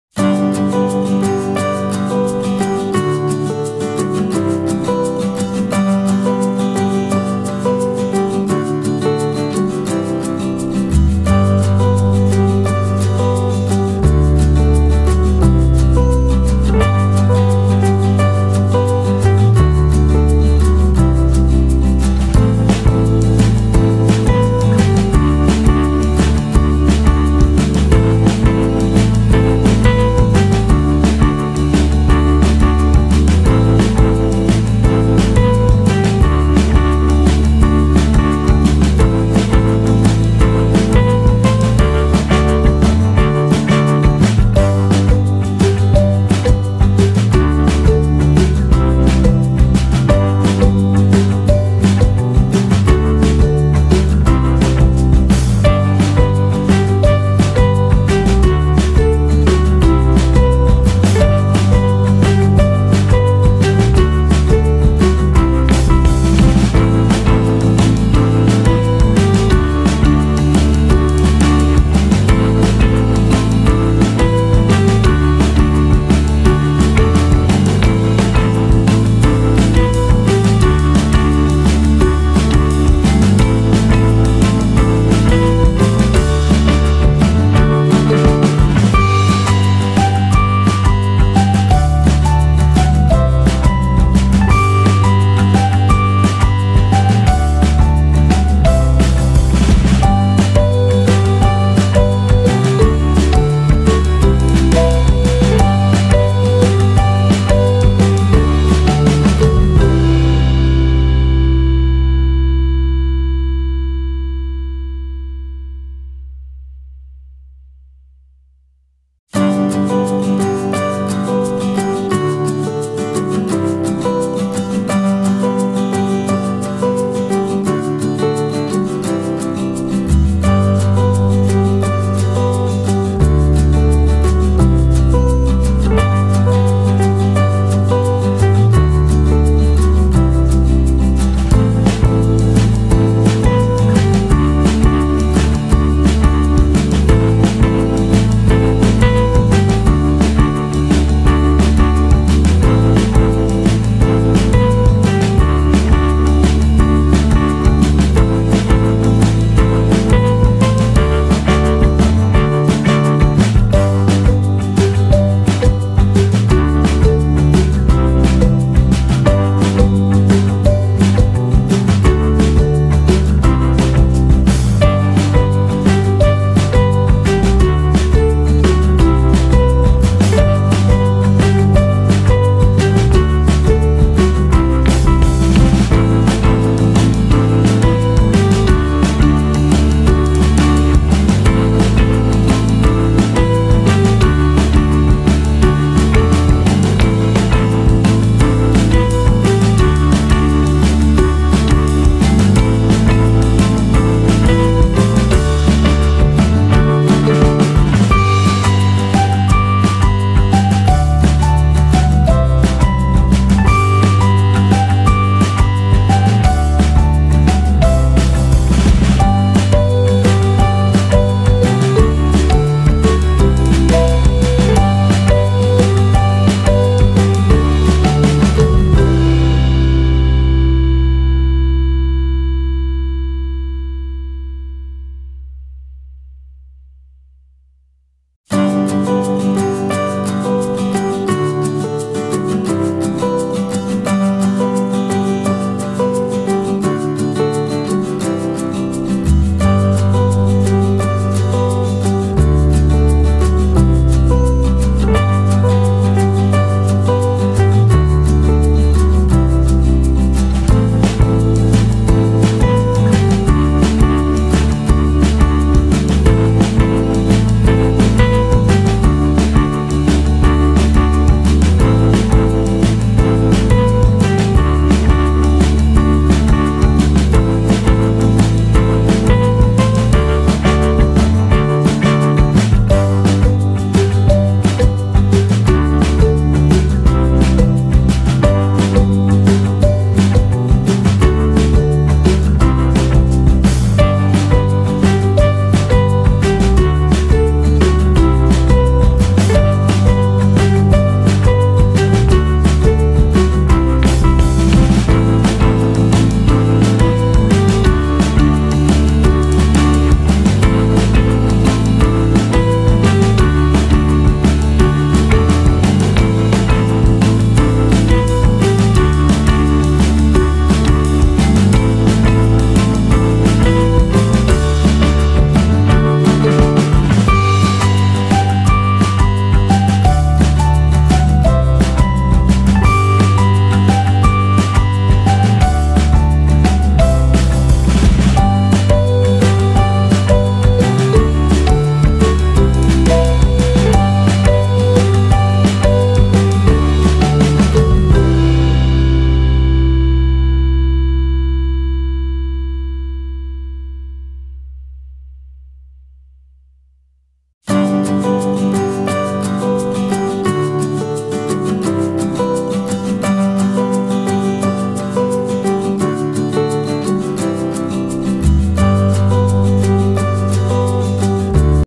Amazing Woolen Craft Ideas - Easy Heart Making with Finger - DIY Wool Design -How to Make Yarn Heart
Easy heart making idea with fingers.

DIY woolen design.